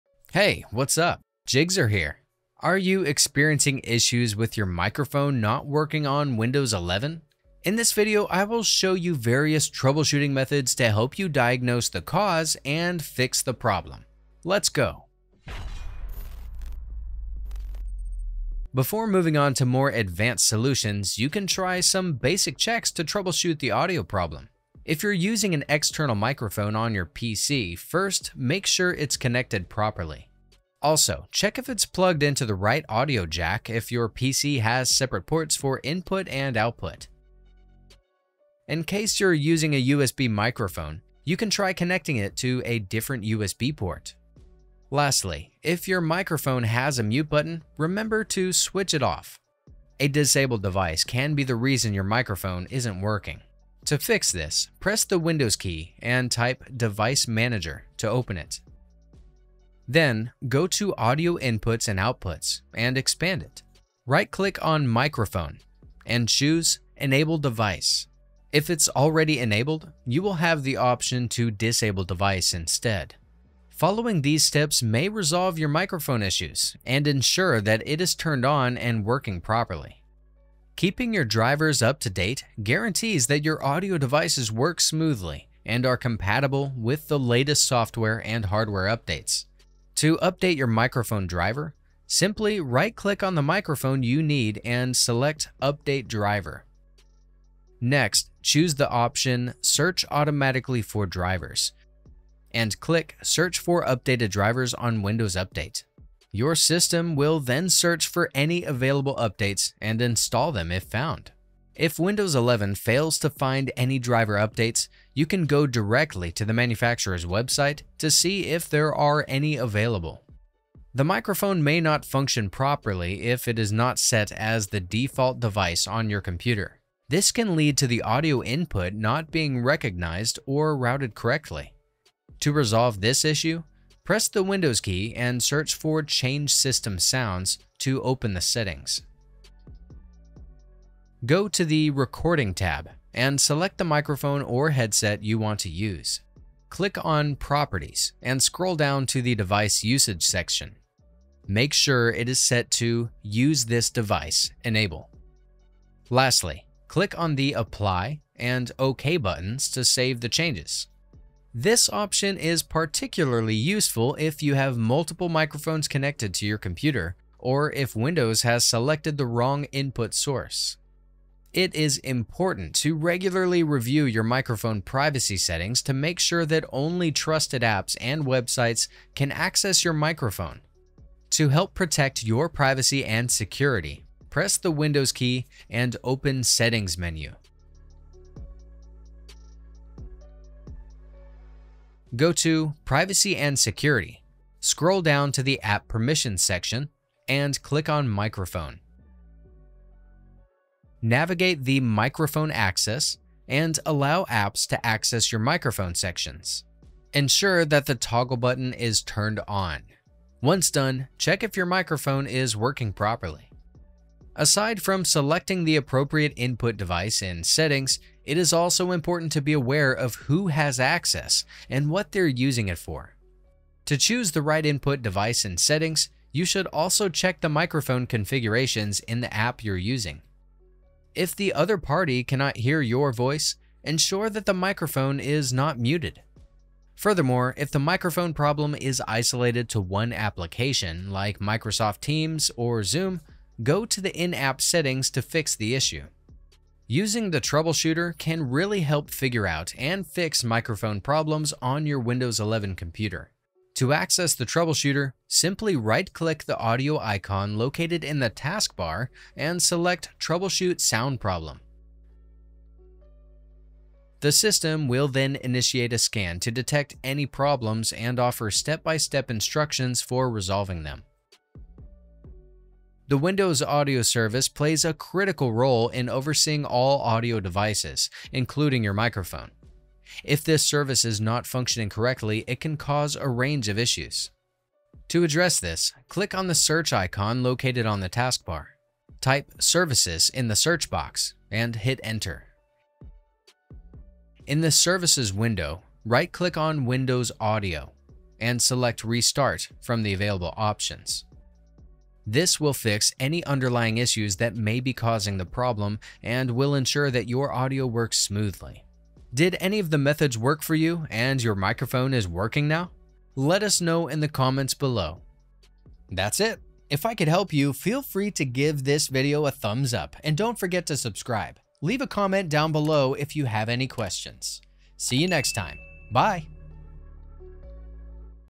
Fix Microphone Not Working on Windows 11! [2024]
Whether you're in a virtual meeting, gaming, or recording content, a malfunctioning microphone on Windows 11 can be a real buzzkill. From no audio input to muffled sound or a complete microphone blackout, we understand the frustration. In this video, we'll I’ll walk you through step-by-step solutions for common microphone problems like no sound, low volume, or unrecognized devices. From checking hardware connections to diving into Windows settings.

Timestamps:
00:00 | Basic Checks
00:49 | Check if microphone is enabled
01:20 | Update related drivers
02:00 | Check the default device
02:51 | Inspect Privacy Settings
03:34 | Wrong microphone settings in apps
04:06 | Run the audio troubleshooter
04:32 | Restart the windows audio service

Video: 677